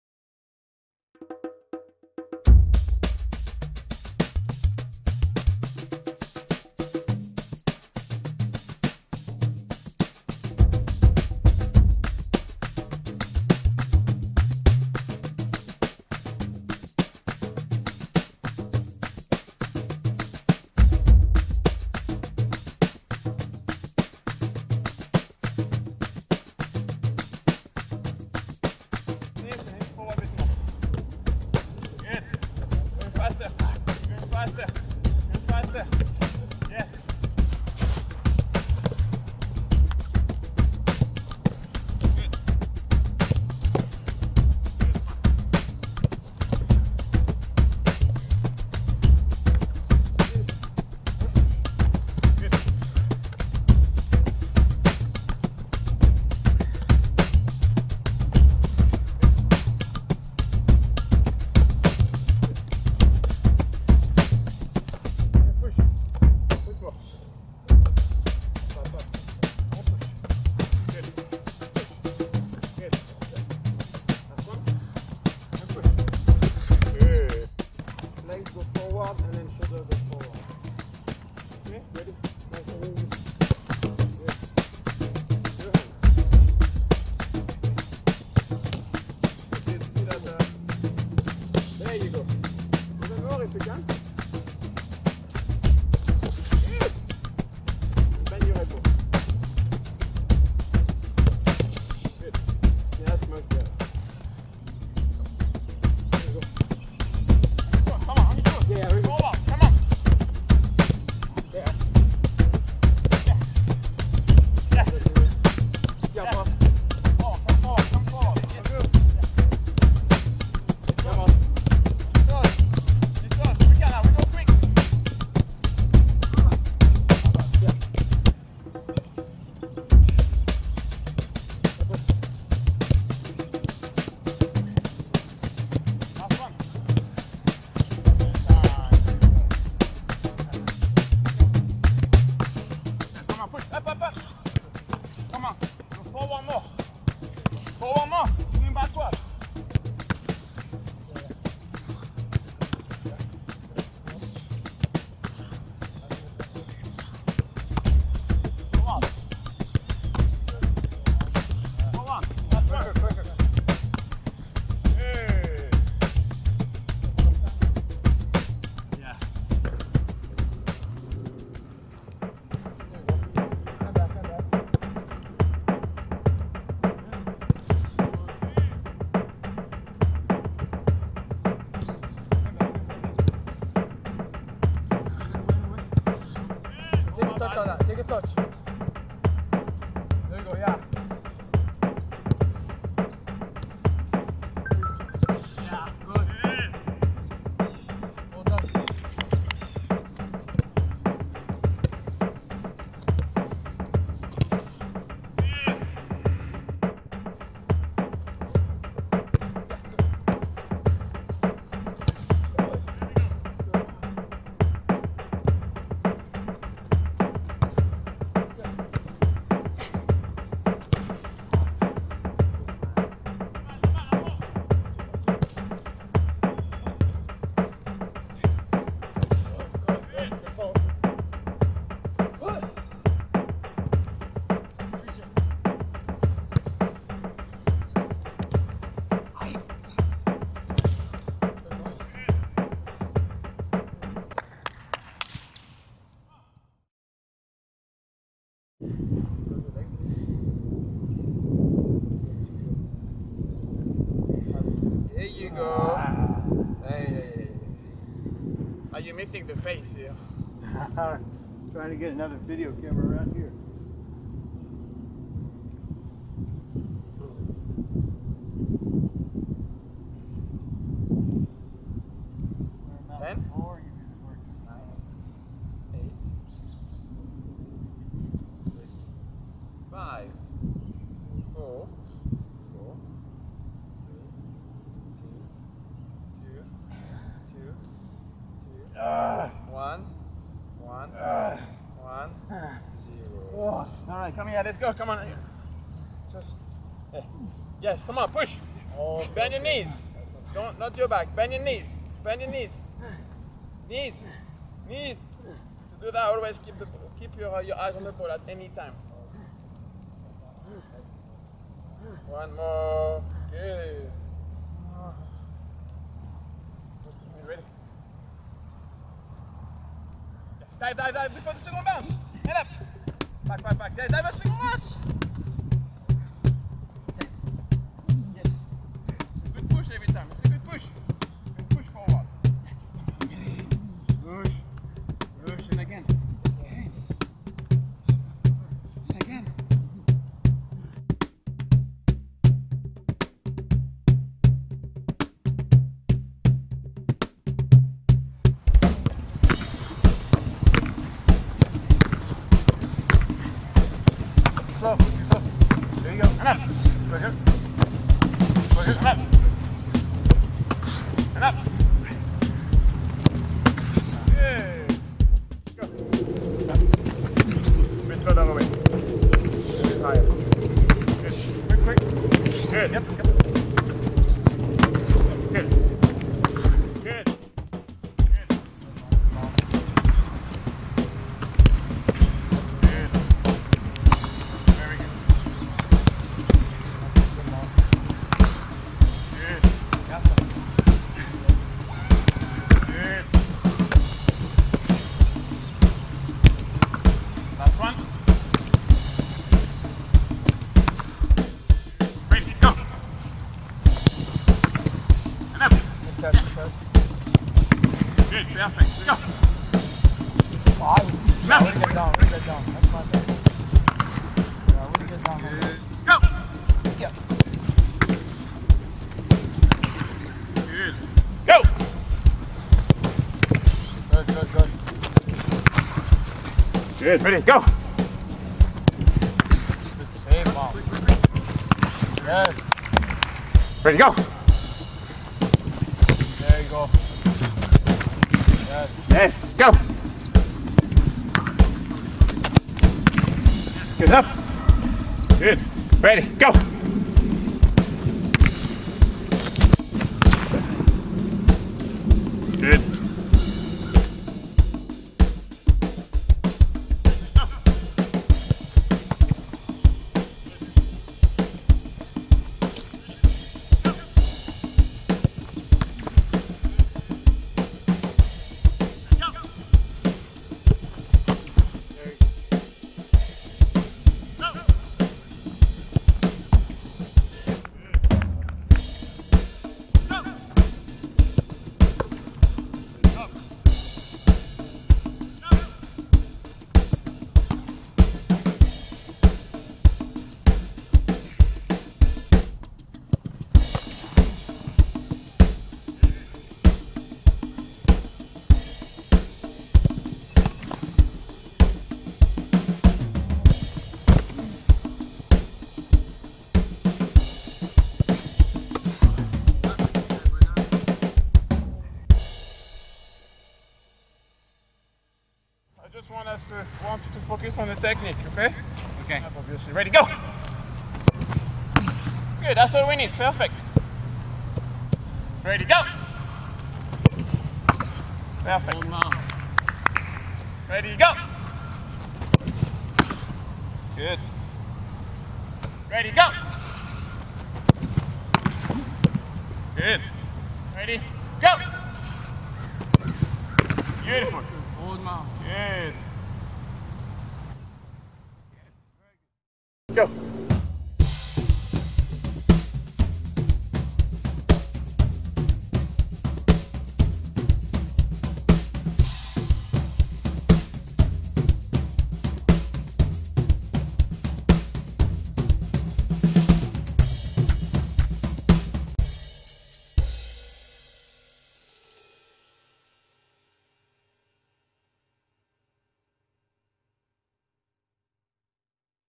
Myles Fullen -Goalkeeper Training Highlights
GK training with Francois DuBourdeau